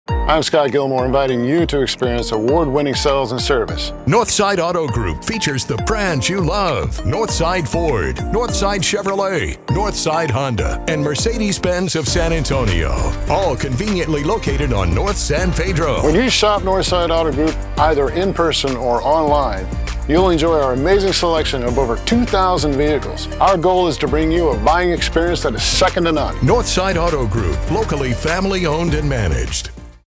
Second To None! The Northside Auto Group Welcomes You
Northside Ford, Northside Honda, Northside Chevrolet, and Mercedes-Benz of San Antonio! All conveniently located on San-Pedro with award-winning customer service for you!